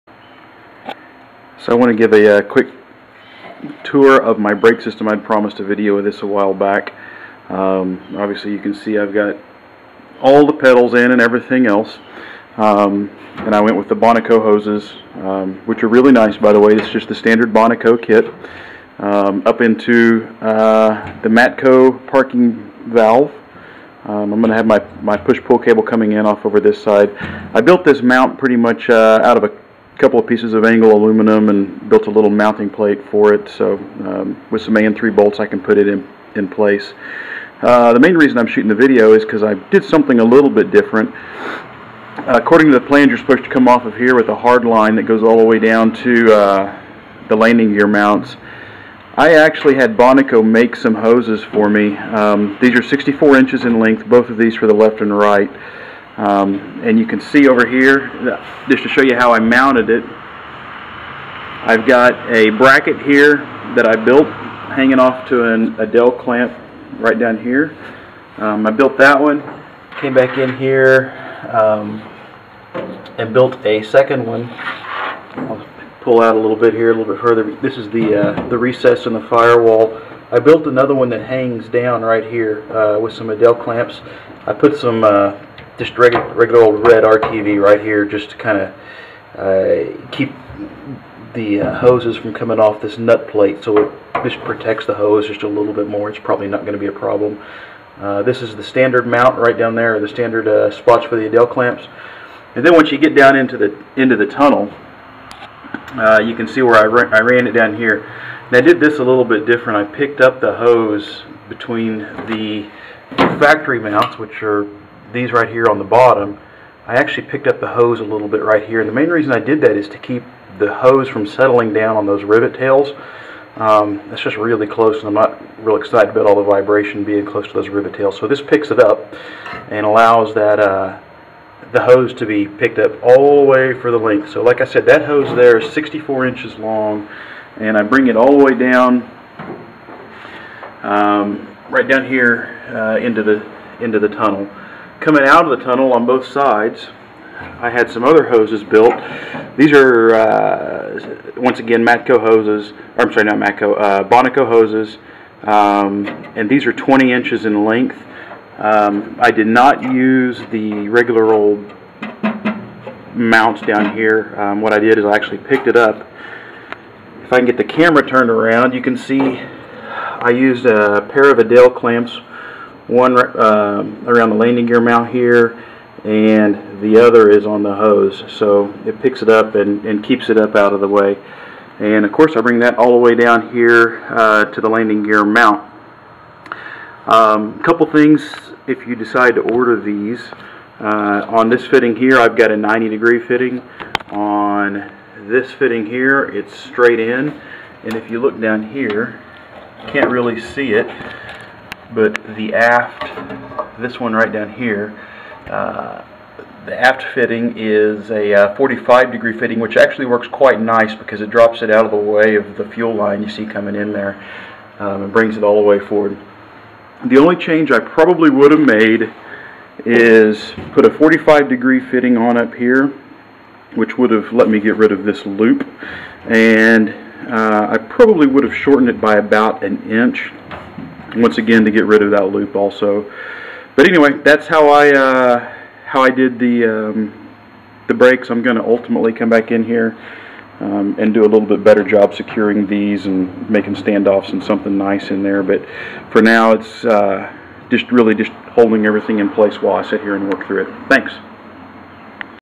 Bonaco Brake Lines - RV-10
This video describes my full installation of Bonoco brake lines at the rudder pedals, through the center tunnel, and to the landing gear mounts